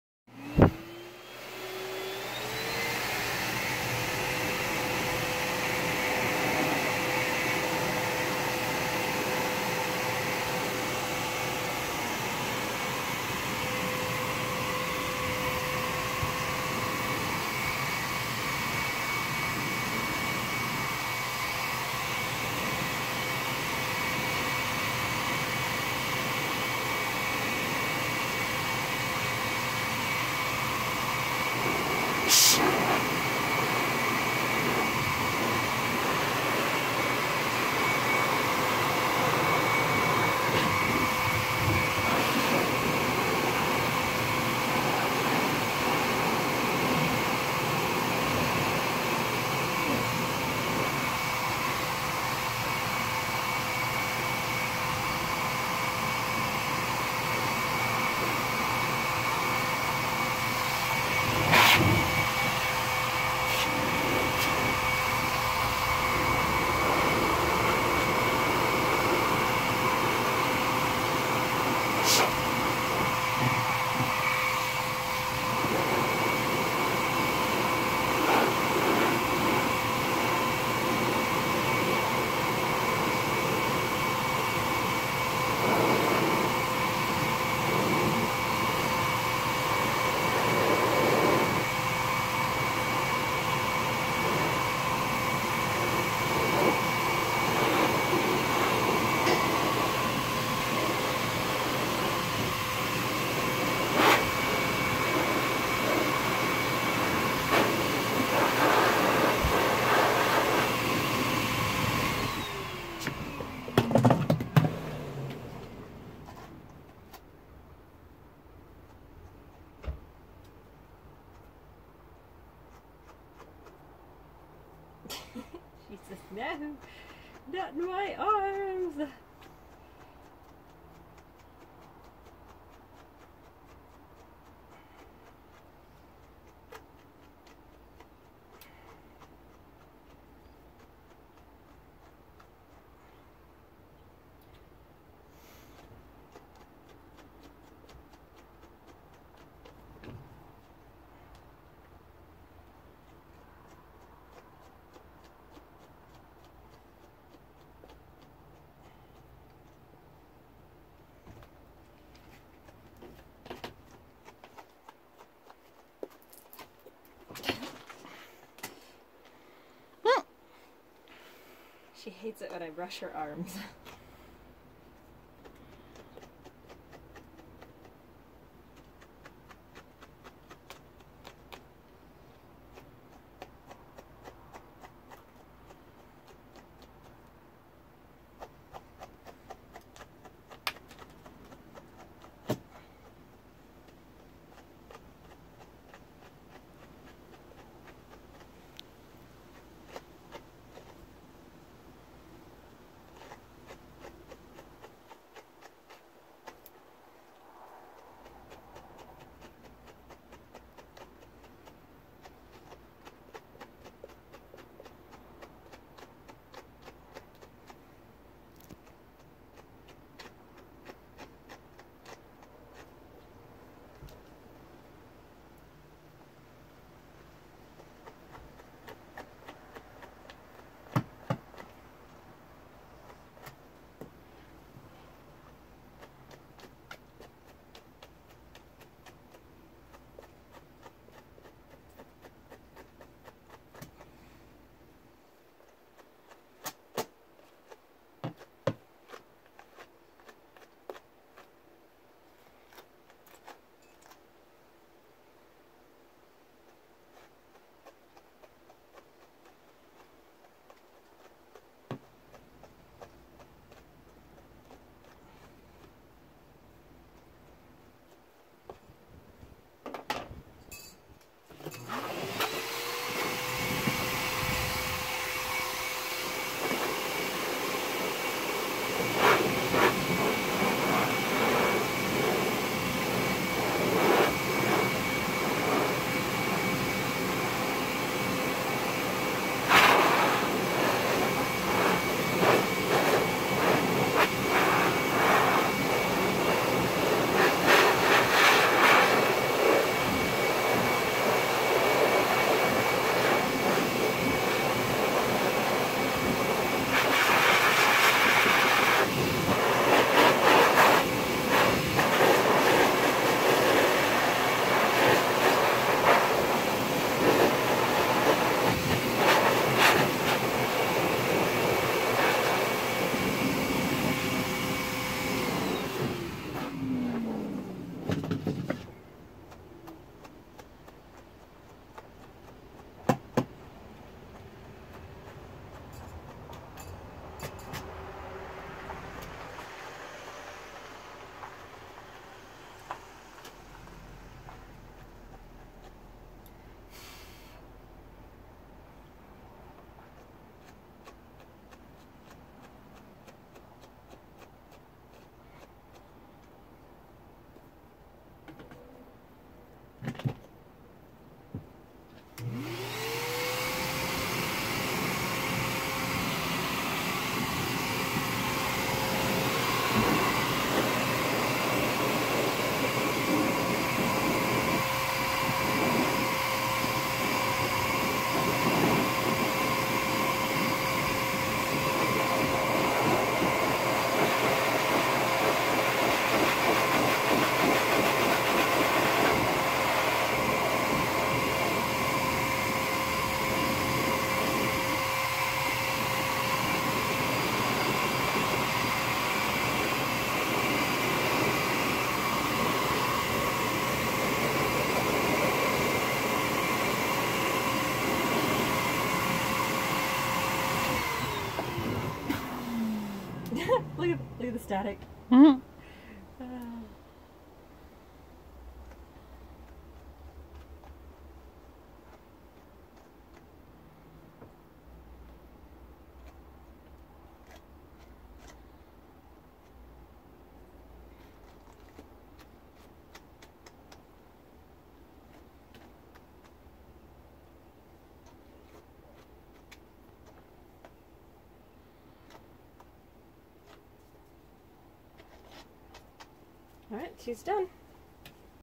Grooming an Angora Rabbit
Grooming an Angora rabbit featuring Stephany Brundage and Tuna.
Don't mind the ice chest, it's the perfect height for blowing so that's what we use. We usually have a carpet on top but it got dirty and is being washed.

steps:
general blow out
flip over and blow out the belly and legs
groom the wool only where it is tangled or matted and where the tips of the fur is dirty or wet
blow out belly again
review with brush again
thoroughly blow out top
shake the air nozzle on certain areas to remove dander and dust